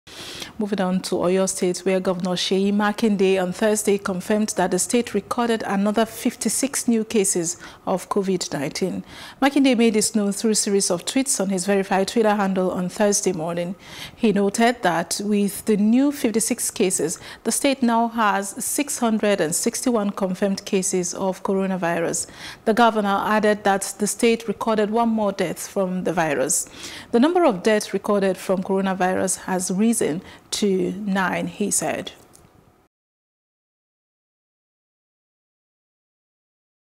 Coronavirus In Oyo: State Records 56 New Cases, One More Death
Governor Seyi Makinde of Oyo State, on Thursday, confirmed that the State recorded another fifty-six (56) new cases of COVID-19.

Makinde made this known through series of tweets on his verified Twitter handle ON Thursday morning.